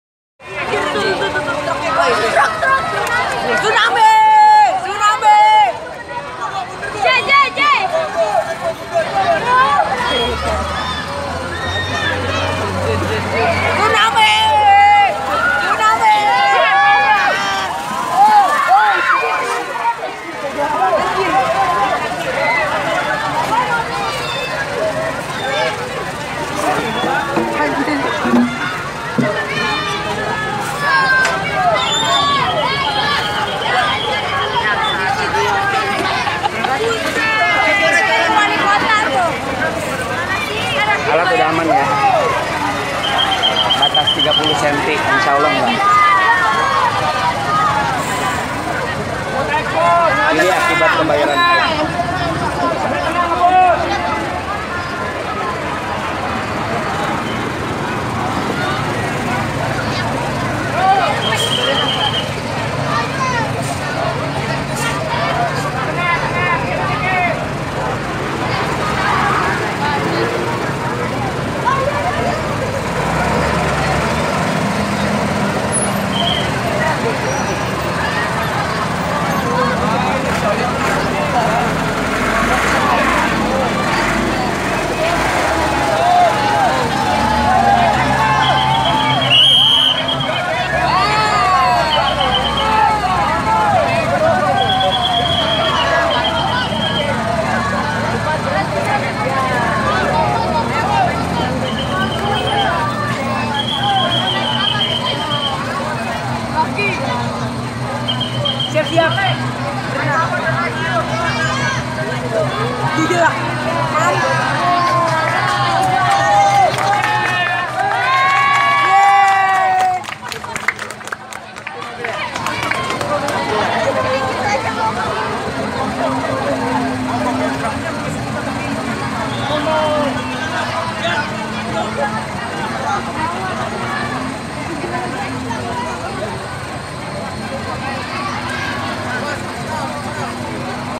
Banjir Pondok Aren Tangerang Selatan
Ada SEBAB ada AKIBAT
Sebelumnya tak pernah terjadi banjir separah ini.